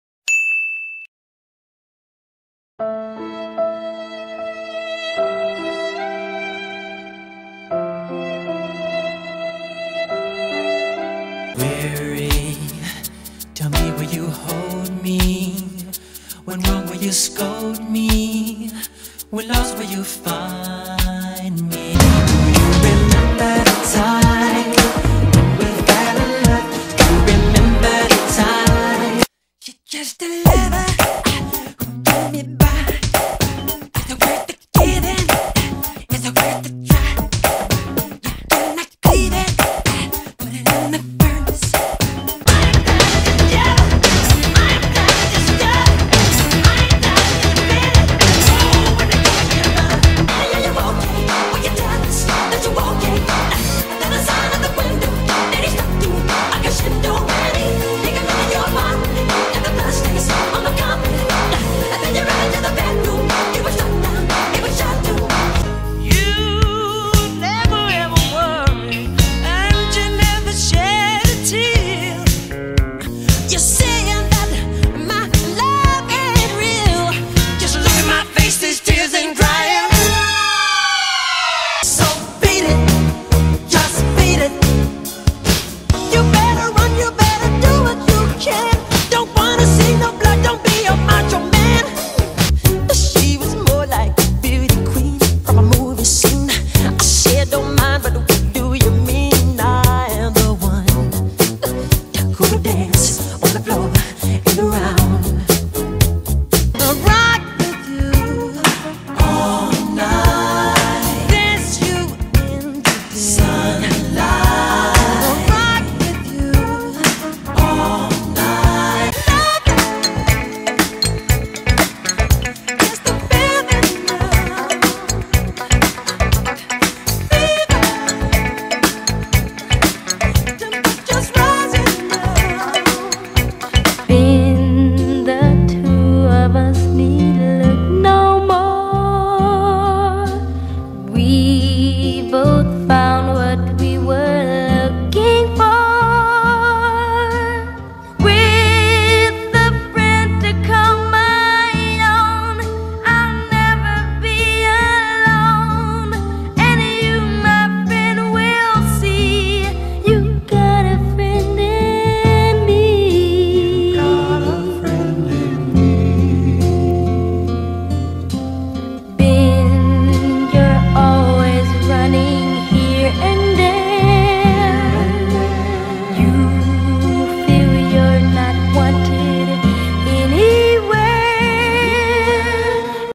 Michael Jackson through the years
R.I.P Michael Jackson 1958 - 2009. He deserves another chance at life.